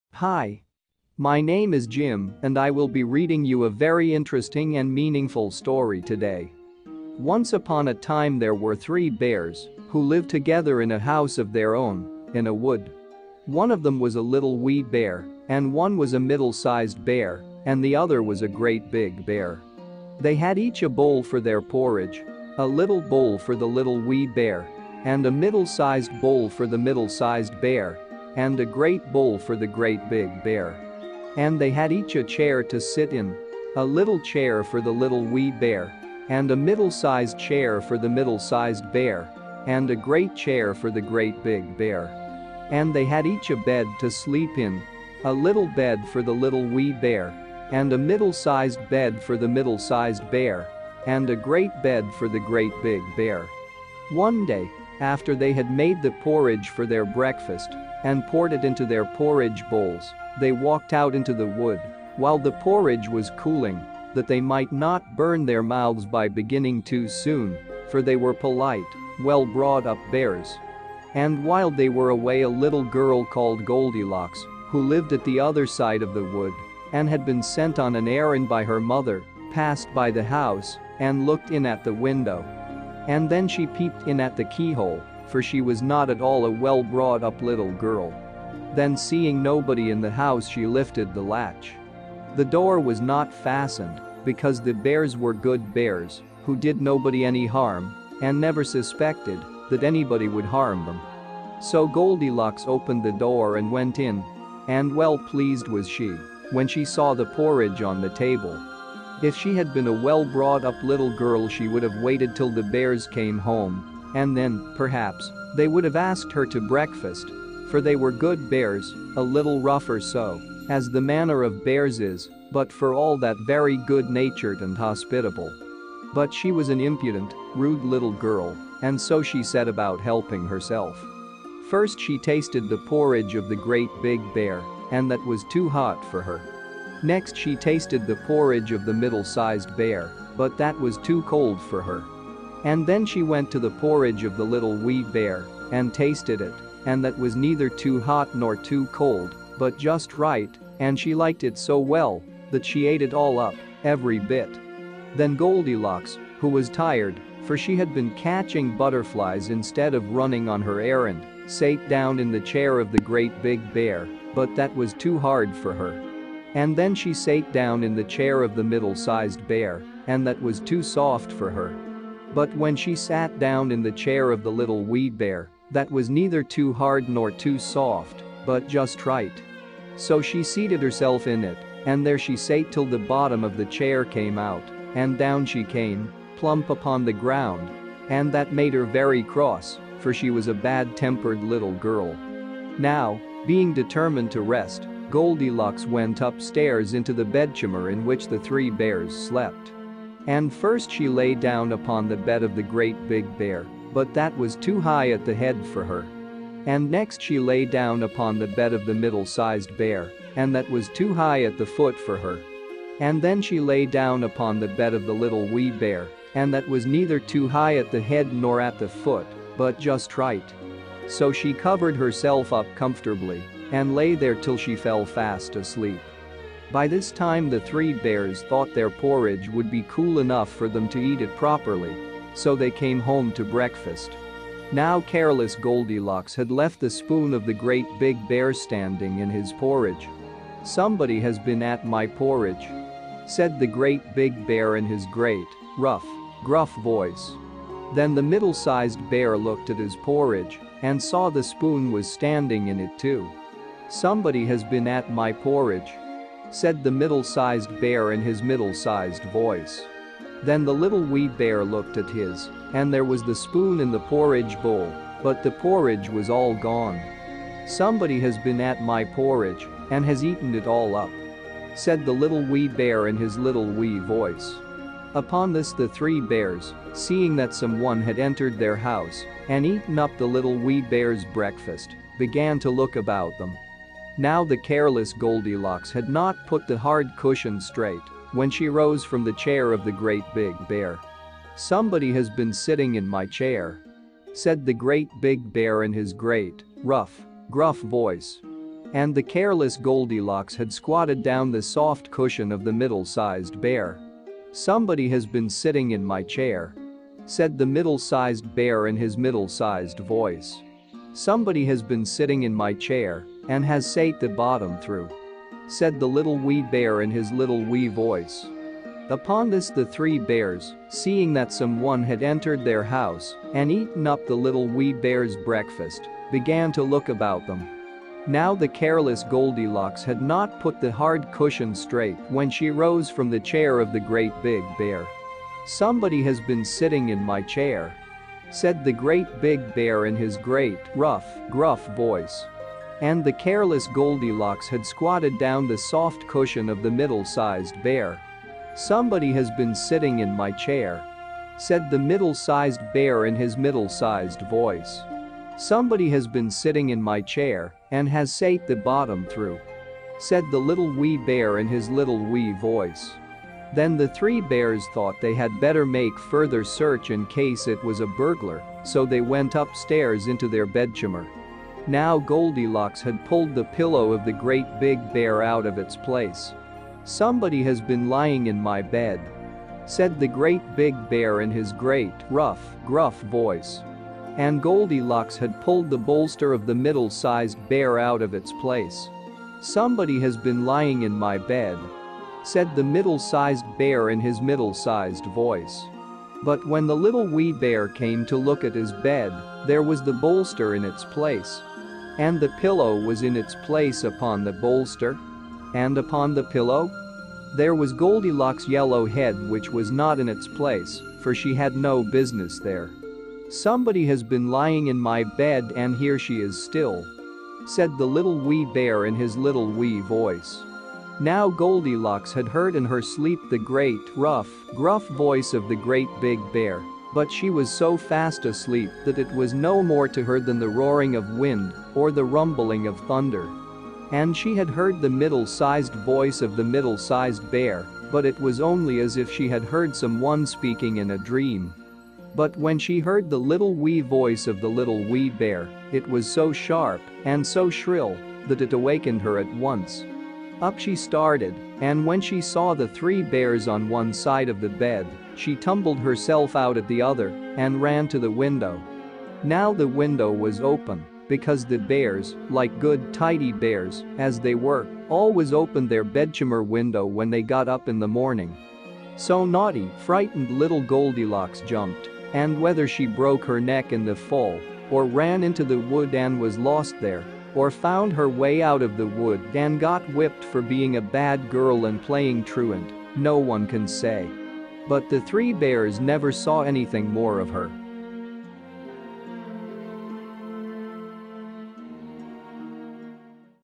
Cuddles | Goldilocks And The Three Bears | Bedtime Story (Ages 4-7)
This story is of a impudent, rude little girl Goldilocks and a family of three bears. Goldilocks and the Three Bears was originally titled "The Story of the Three Bears", published in the collection English Fairytales, retold by Flora Annie Steel (1922).

Recommended for ages 4-7.